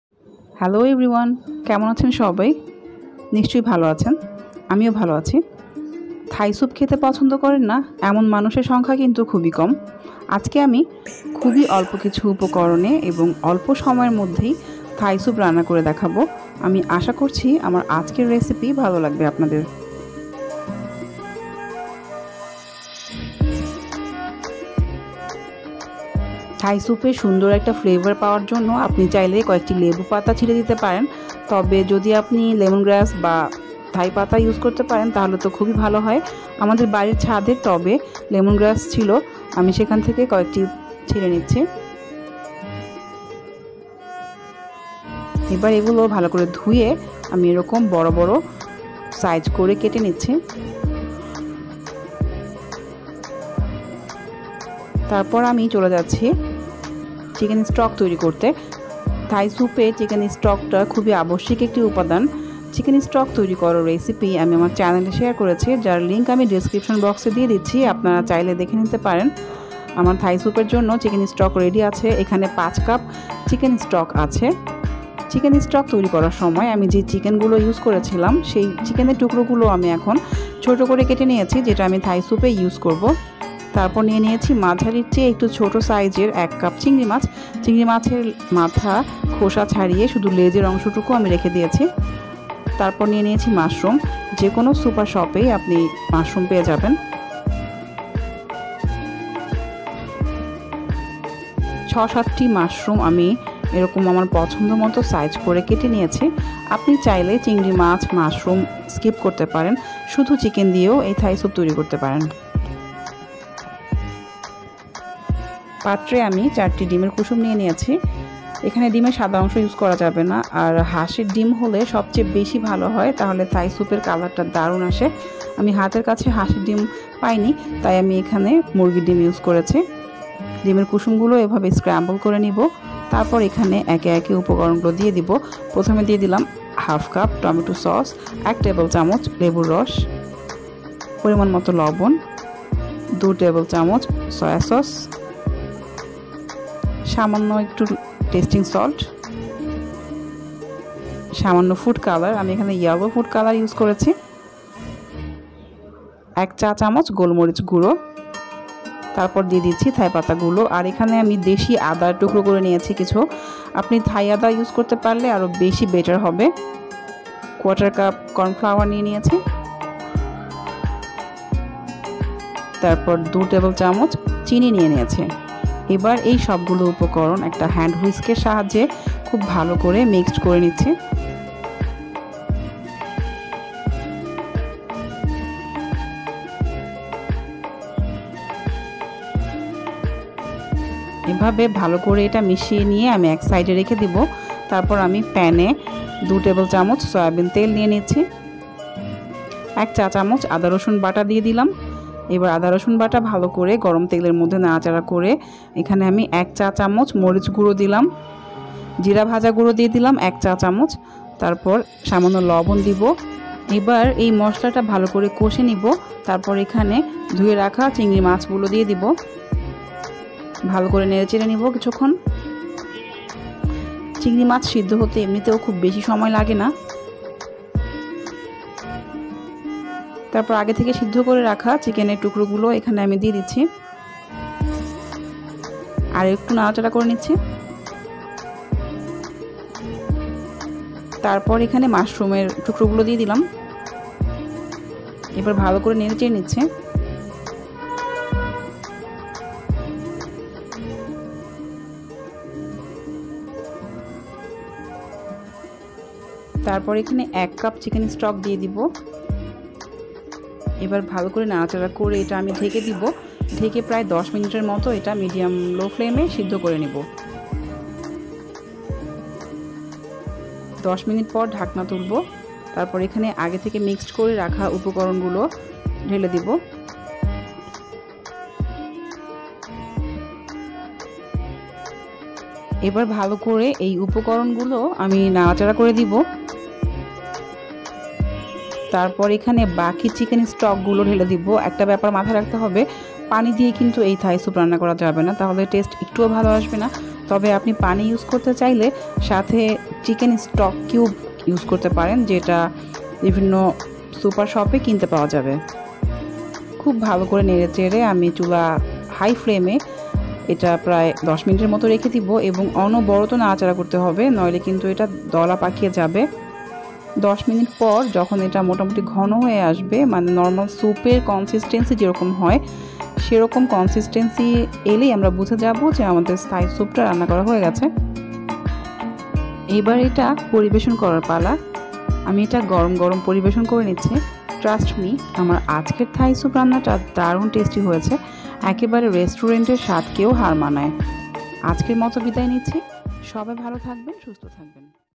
২০ মিনিটে সহজ এবং দারুণ স্বাদের থাই স্যুপ রেসিপি||Easy and Tasty Thai Soup Recipe in 20 minutes
চিকেন স্টক রেসিপি লিংক---